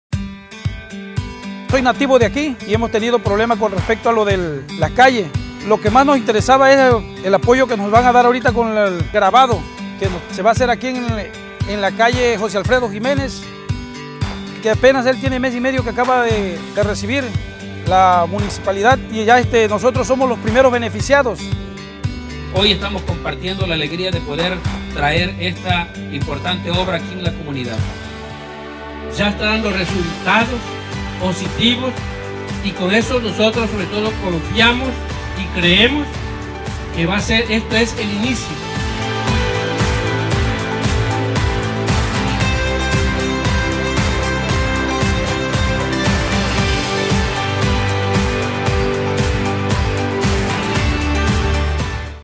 Calle José Alfredo Jiménez, Ra Pueblo Viejo, Jalpa de Méndez
Construcción de la Base Hidráulica, con Grava de Revestimiento de la calle Calle José Alfredo Jiménez, Ra  Pueblo Viejo, Jalpa de Méndez